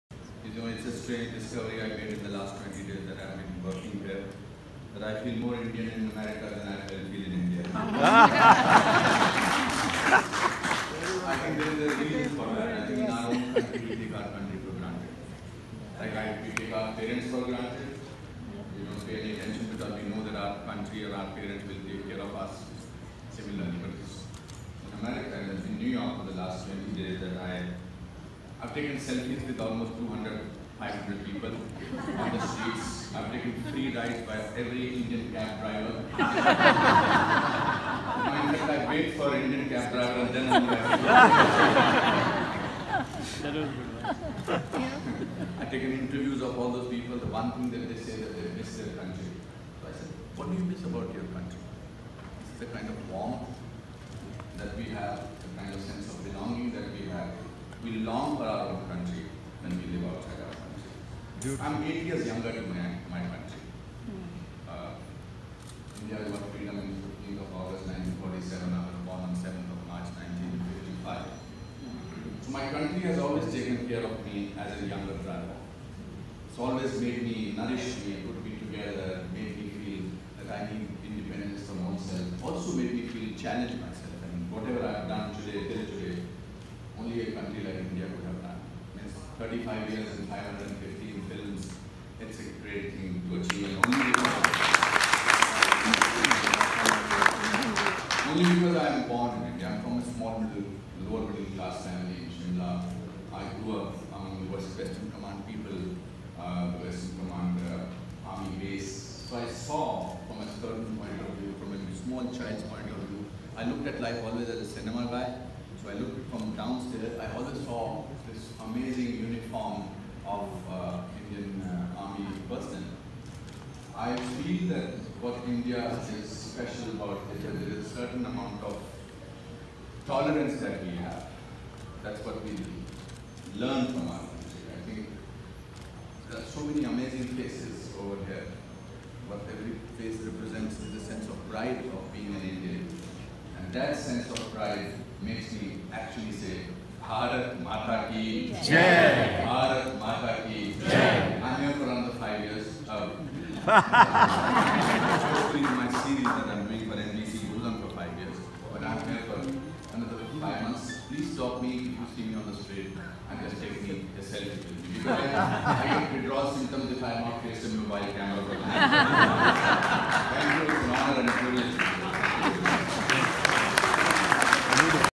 Anupam Kher addresses NYC Crowd on the Independece day at Indian Consulate
Anupam Kher addresses NYC Crowd on the Independece day